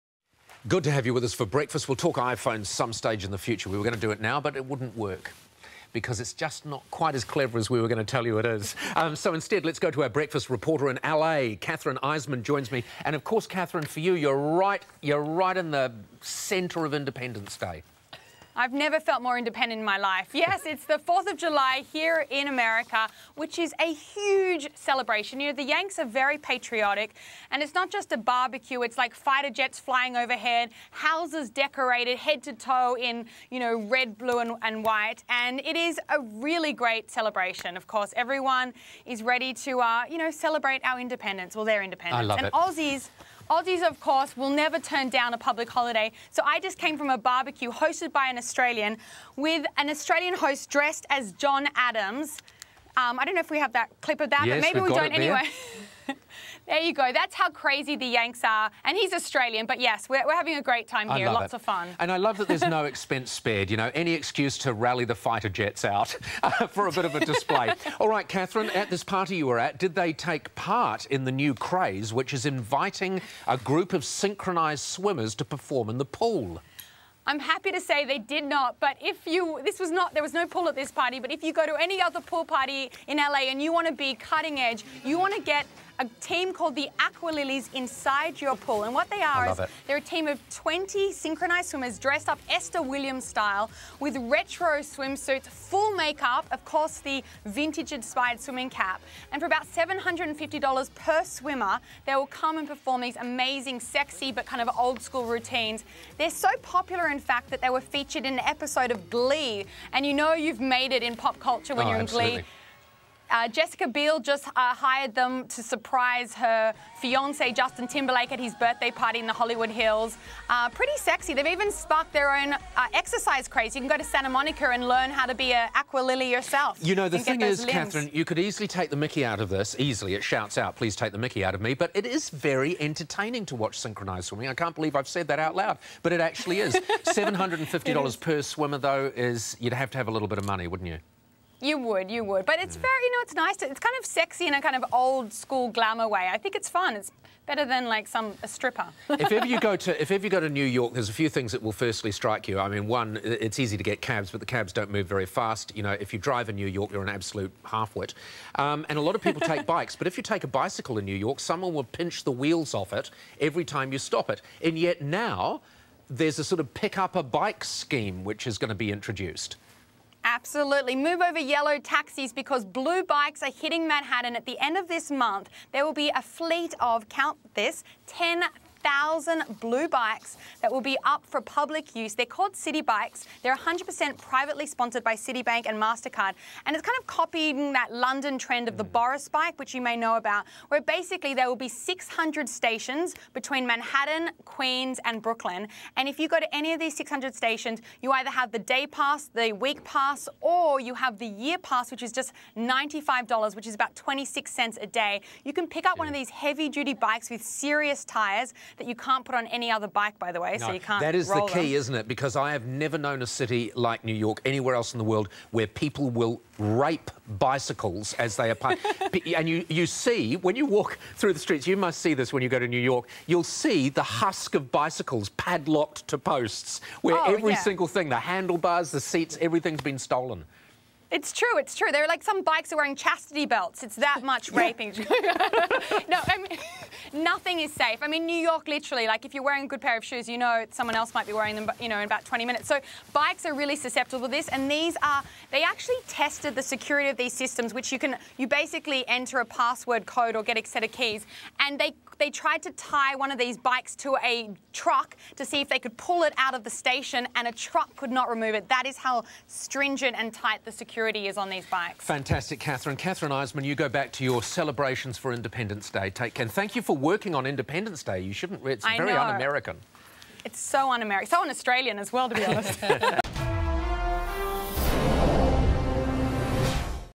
4th July
Paul Henry chats with Kathryn Eisman about the 4th of July.